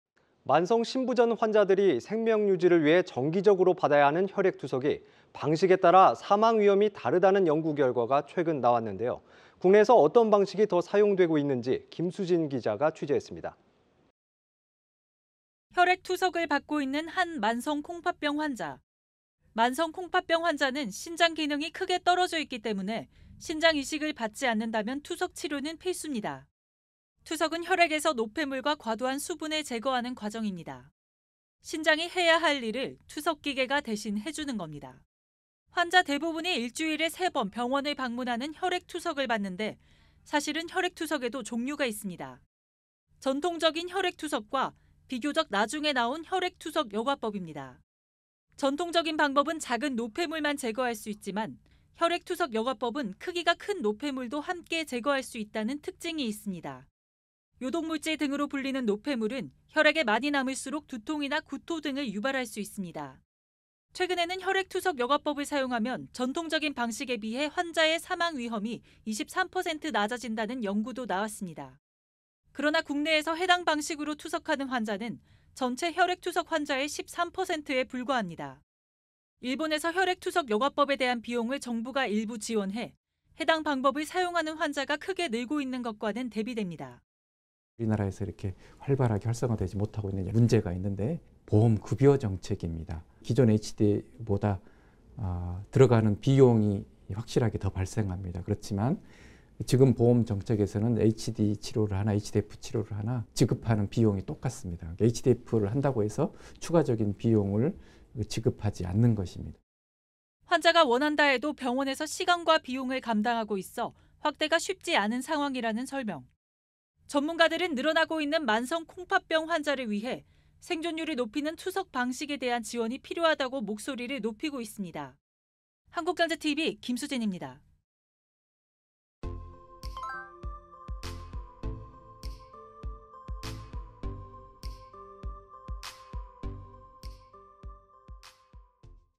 투석은 다 똑같다? "방식따라 사망 위험 23% 차이" [건강팁스터]/한국경제TV뉴스
만성 신부전 환자들이 생명 유지를 위해 정기적으로 받아야 하는 혈액투석이, 방식에 따라 사망 위험이 다르다는 연구결과가 최근 나왔습니다. 산업2부 김수진 기자가 취재했습니다.